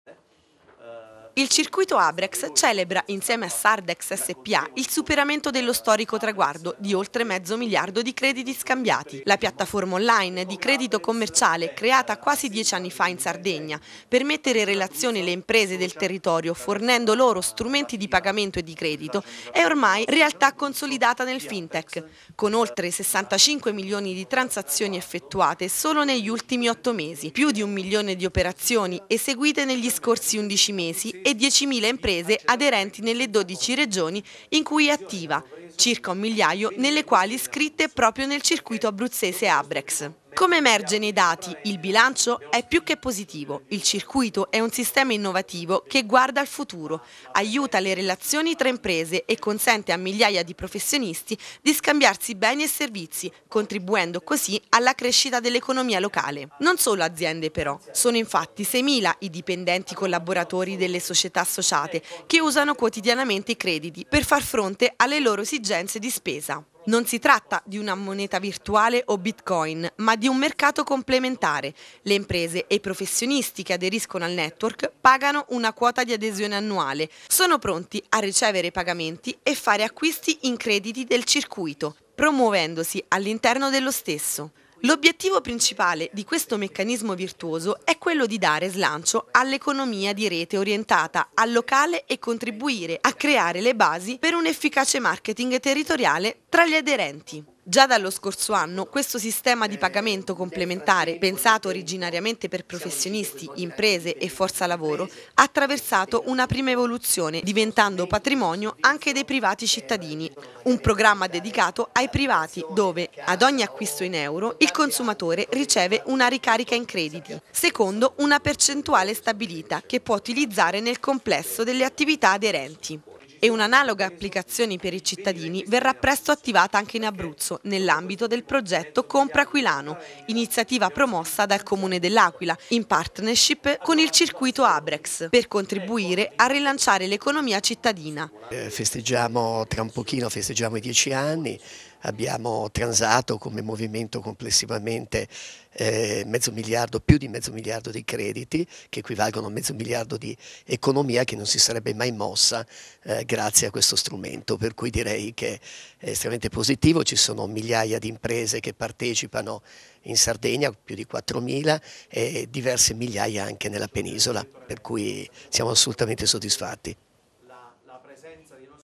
Ottimi risultati per Sardex e Abrex
Il circuito Abrex celebra insieme a Sardex il superamento dello storico traguardo di oltre mezzo miliardo di euro di acquisi e vendite effettuate tramite il circuito. Una piattaforma nata in Sardigna quasi 10 anni fa per mettere in relazione le imprese del territorio.